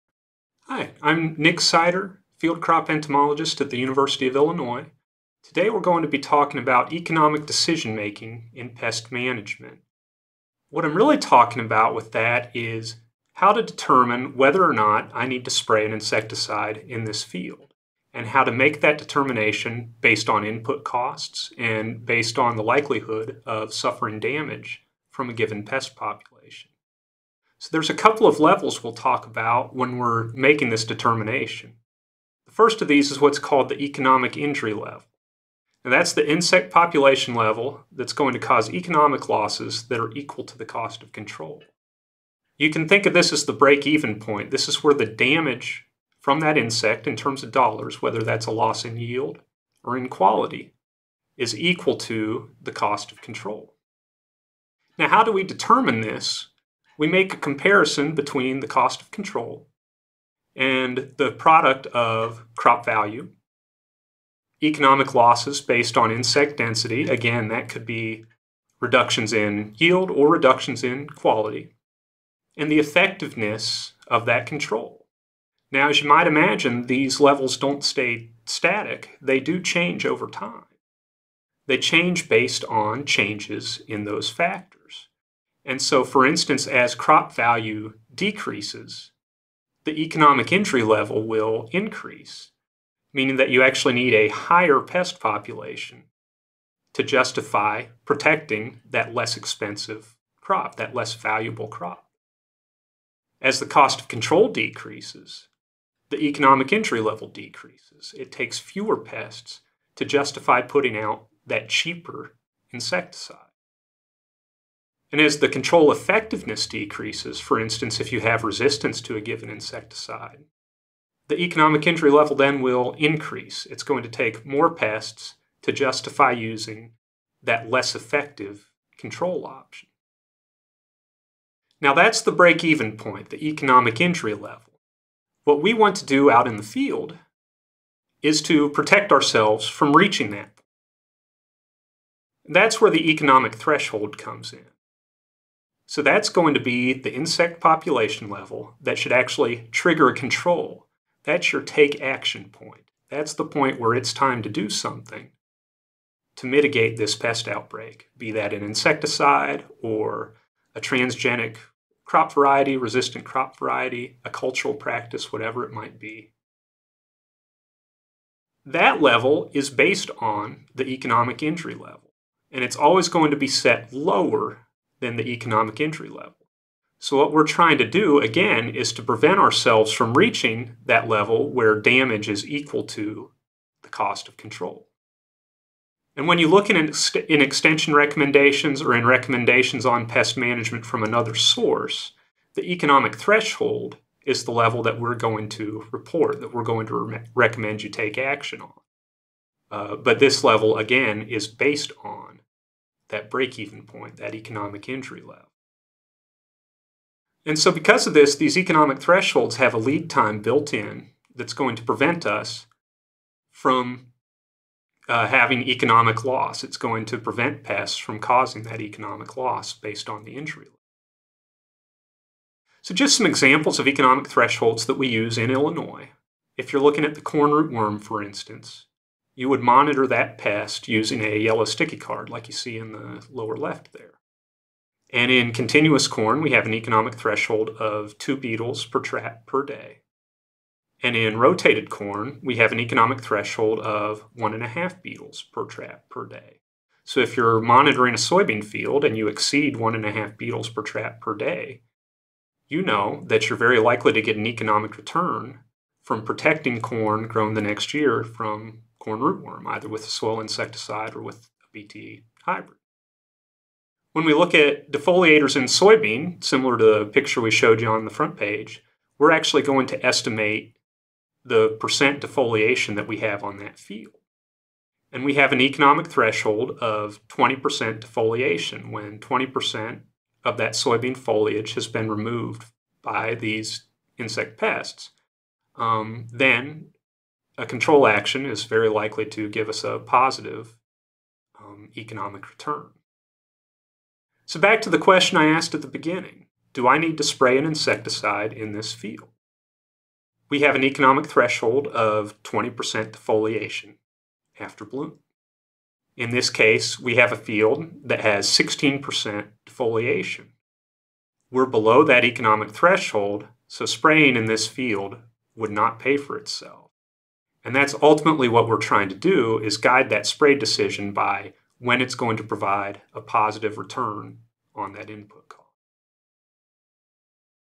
Economic decision making in pest management
Nick Seiter discusses how to use economic injury levels and economic thresholds to guide pest management decisions.